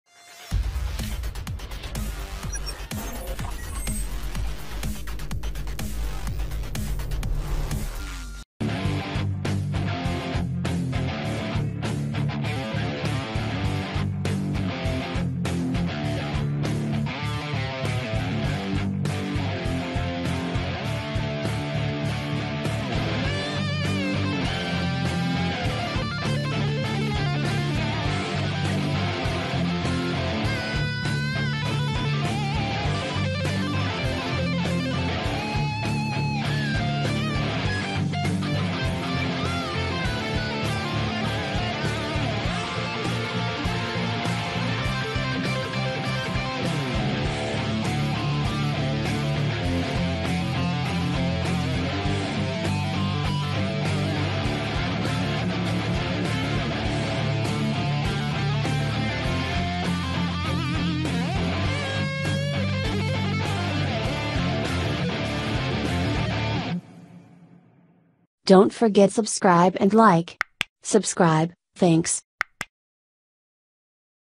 Soloing on electric guitar - Beatiful talented girl
Laura cox, Beatiful talented girl
frensh classic rock band